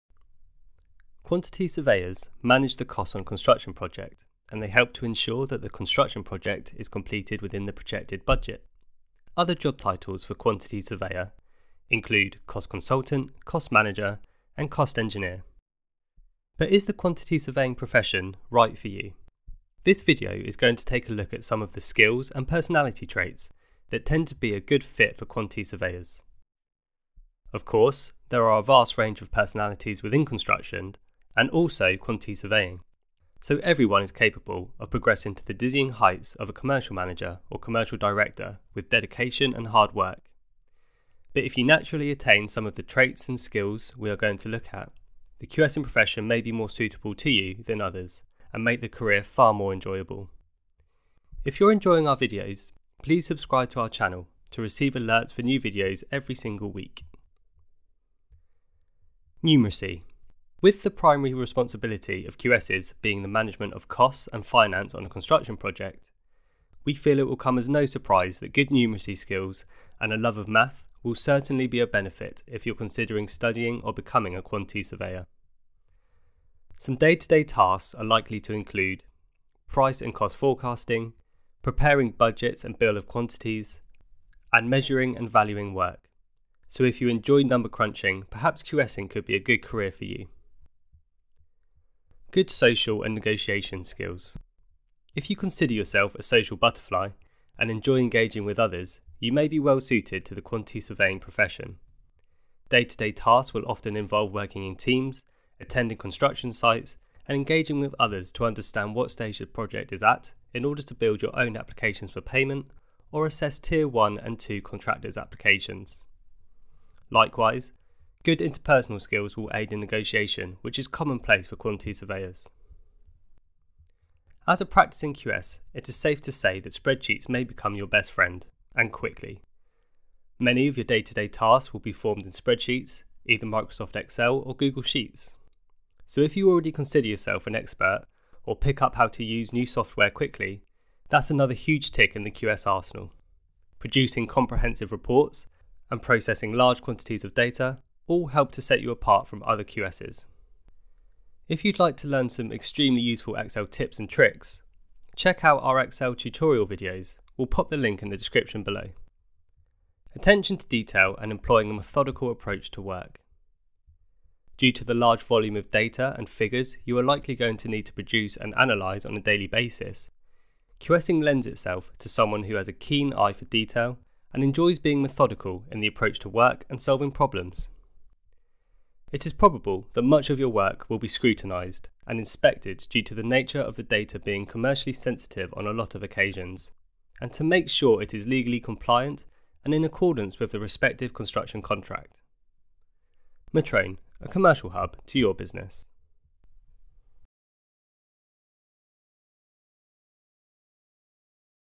Is Quantity Surveying For You?
Quantity Surveyors play a huge role in the management of construction projects and contracts. This video explores some of the personality traits and skills that are suited to this great profession!

Excel Hints and Tips Videos;